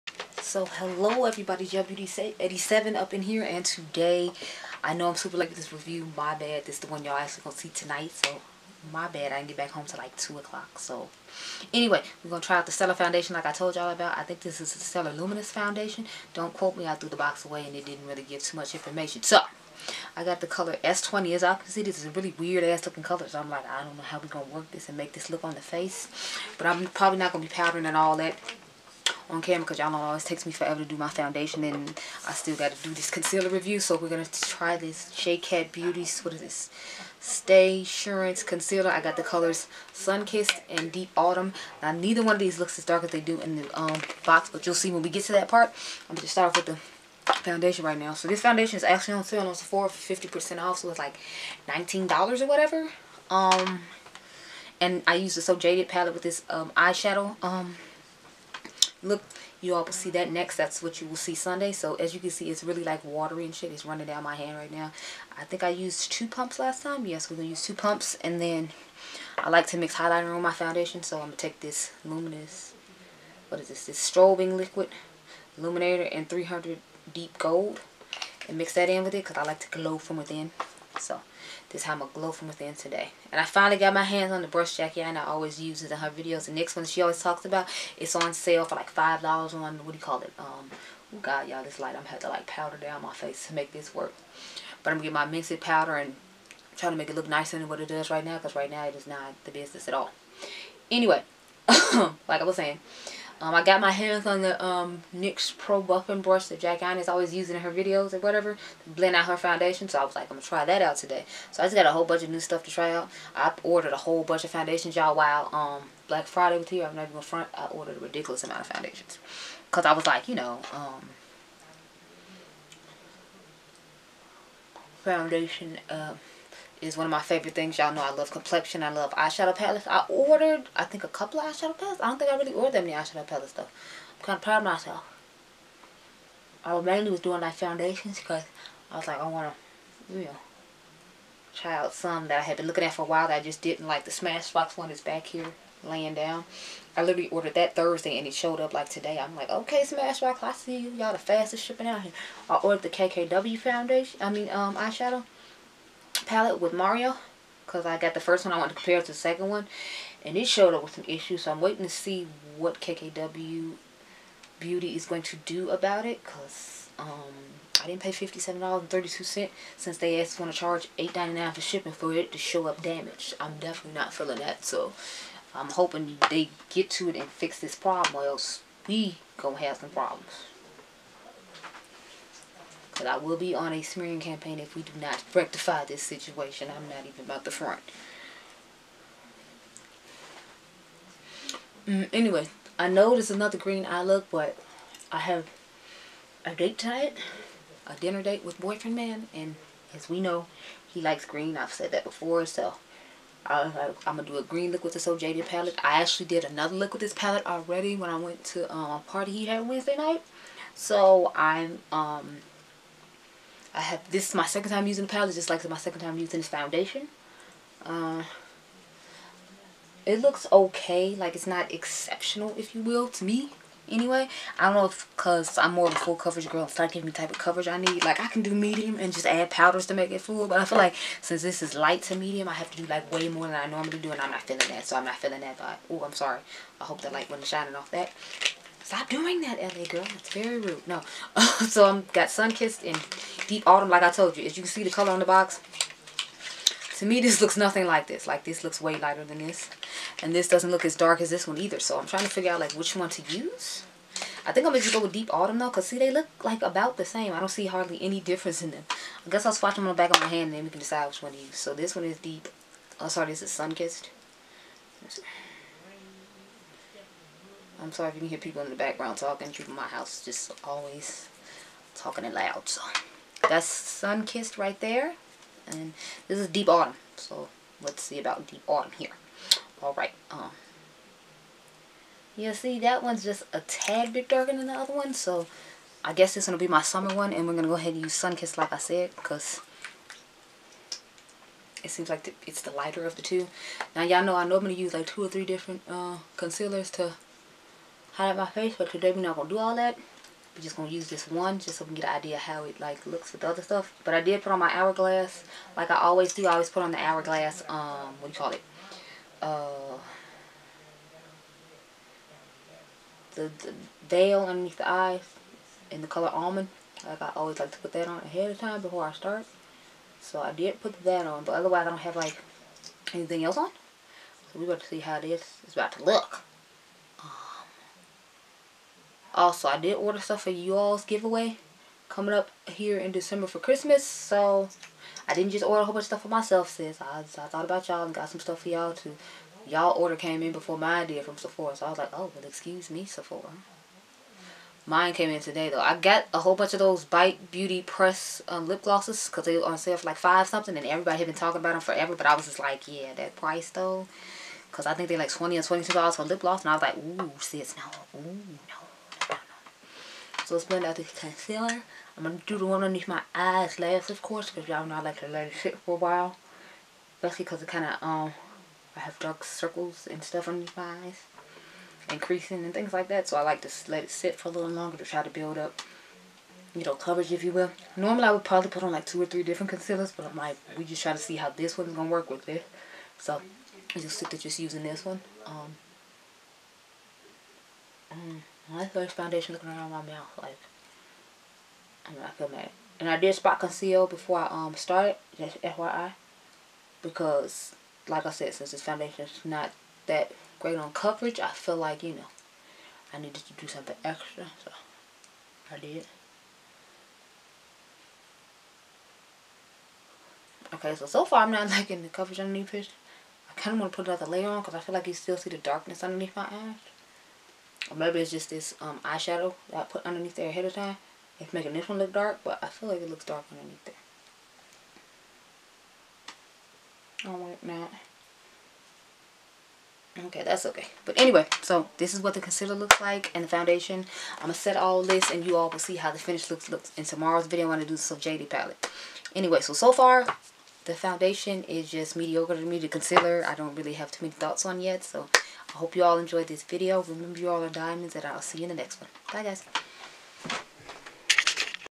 Stellar Luminous Foundation & J. Cat Beauty Stay Surance Concealer
Update: This foundation wore nicely but I feel like it lacks the kind of coverage I like in a foundation. It’s fine for winter time but I would not wear it in the summer time because I feel like it wouldn’t be able to last throughout the day like I would need it to.
Hello Men, Women, Children & Babies! Today I will be trying out the new Stella Luminous Foundation & J. Cat Beauty Stay Surance Concealer. I have been wanting to try both of these products for quite some time and finally caught them at a good enough deal to make me purchase them. So I figured I share my thoughts with you all in a video.  I hope you enjoy.